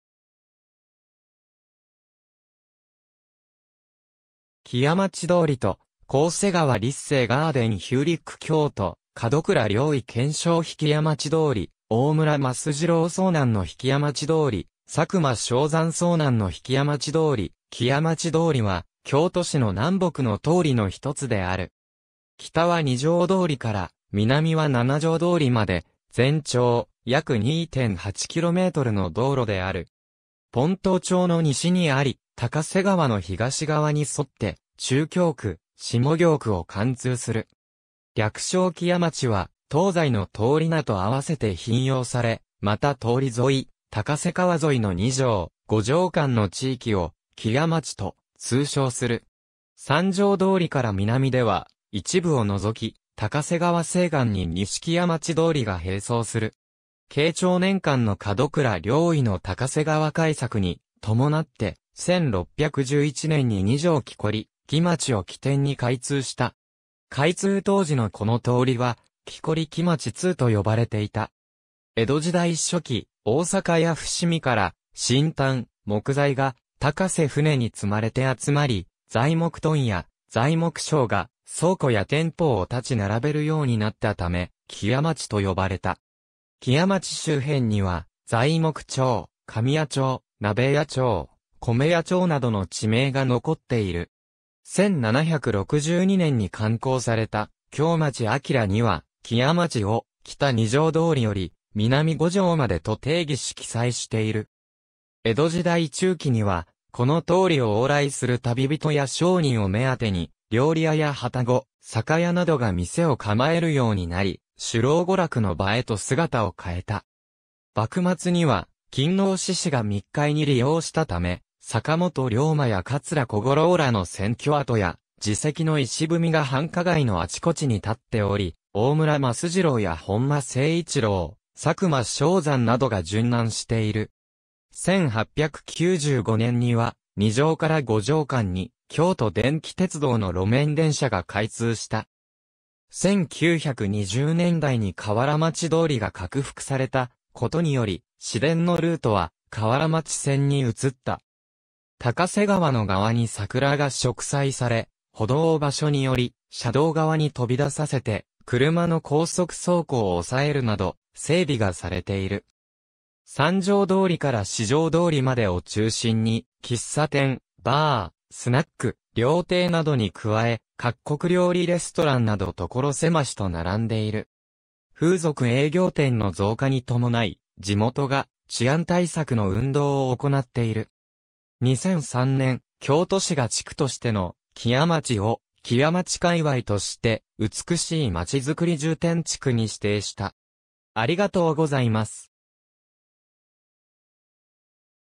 木屋町通
木屋町通と高瀬川 立誠ガーデン ヒューリック京都・角倉了以顕彰碑 木屋町通・大村益次郎遭難の碑 木屋町通・佐久間象山遭難の碑 木屋町通、木屋町通り（きやまちどおり）は、京都市の南北の通りの一つである。
北は二条通から南は七条通まで、全長約2.8キロメートルの道路である。
先斗町の西にあり、高瀬川の東側に沿って中京区・下京区を貫通する。
略称木屋町（きやまち）は、東西の通り名と合わせて頻用され、また通り沿い・高瀬川沿いの二条・五条間の地域を「木屋町」と通称する。
三条通から南では、一部を除き高瀬川西岸に西木屋町通（にしきやまちどおり）が並走する。
慶長年間（1596年 - 1615年）の角倉了以の高瀬川開削に伴って、1611年（慶長16年）に二条樵木町（現在の中京区上樵木町付近）を起点に開通した。
開通当時のこの通りは、「樵木町通」と呼ばれていた。
江戸時代初期（17世紀）、大坂（現在の大阪府大阪市）や伏見（現在の京都市伏見区）から薪炭・木材が高瀬舟に積まれて集まり、材木問屋・材木商が倉庫や店舗を立ち並べるようになったため、「木屋町」と呼ばれた。
木屋町周辺には材木町、紙屋町、鍋屋町、米屋町などの地名が残っている。
1762年（宝暦12年）に刊行された『京町鑑』には、「木屋町」を「北二条通より南五条迄」と定義し記載している。
江戸時代中期（18世紀）には、この通りを往来する旅人や商人を目当てに、料理屋や旅籠、酒屋などが店を構えるようになり、酒楼娯楽の場へと姿を変えた。
幕末（19世紀）には勤皇志士が密会に利用したため、坂本龍馬や桂小五郎らの潜居跡や事跡の碑が繁華街のあちこちに立っており、大村益次郎や本間精一郎、佐久間象山などが殉難している。
1895年（明治28年）には、二条 - 五条間に京都電気鉄道（のちに買収され京都市電木屋町線）の路面電車が開通した。
1920年代（大正末年 - 昭和初年）に河原町通が拡幅されたことにより、市電のルートは河原町線に移った。
高瀬川の側にサクラが植栽され、歩道を場所により車道側に飛び出させて車の高速走行を押さえるなど、整備がされている。
三条通から四条通までを中心に、喫茶店、バー、スナック、料亭などに加え、各国料理レストランなど所狭しと並んでいる。
風俗営業店の増加に伴い、地元が治安対策の運動を行っている。
2003年（平成15年）、京都市が地区としての「木屋町」を、「木屋町界隈」（きやまちかいわい）として「美しいまちづくり重点地区」に指定した。